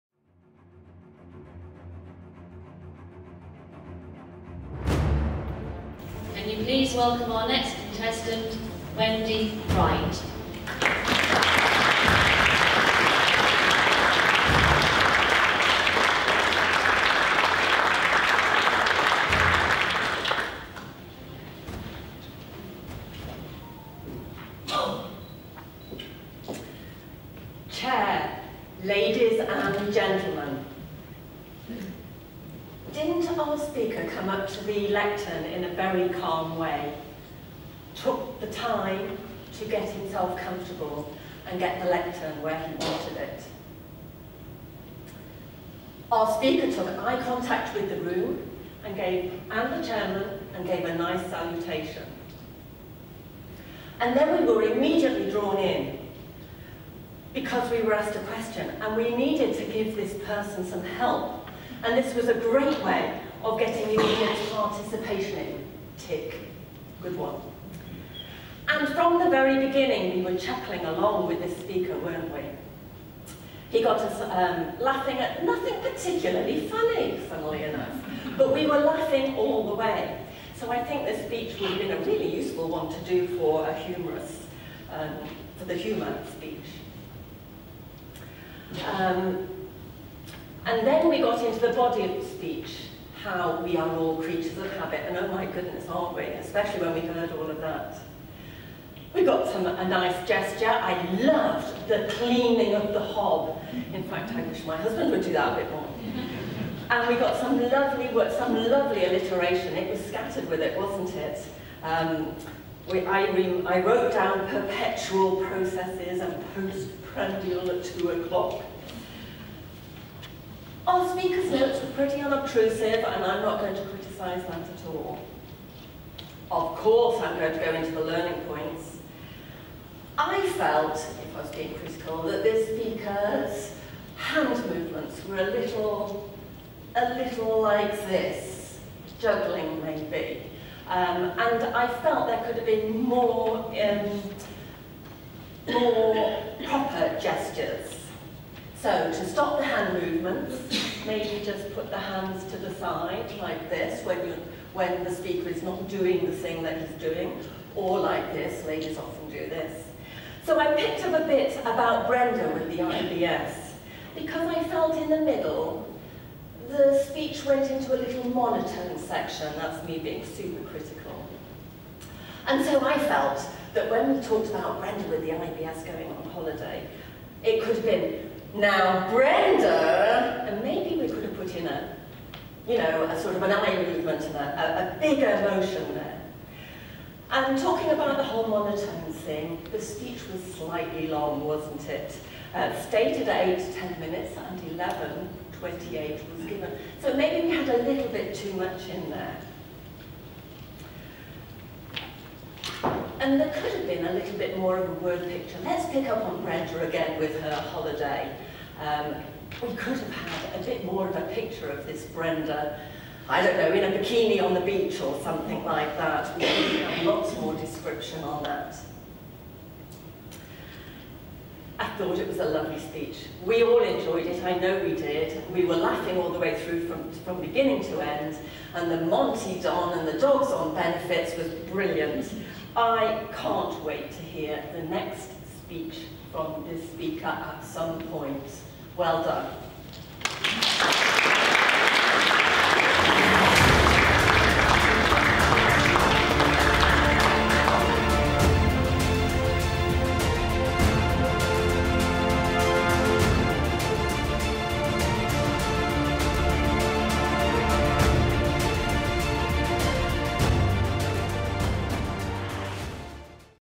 Speech Evaluation Competition - Wendy Wright
46th Annual Conference 2017
Location: The Hilton Hotel Gatwick 21 - 23 April 2017

The ASC - Where you become a better speaker, presenter and leader.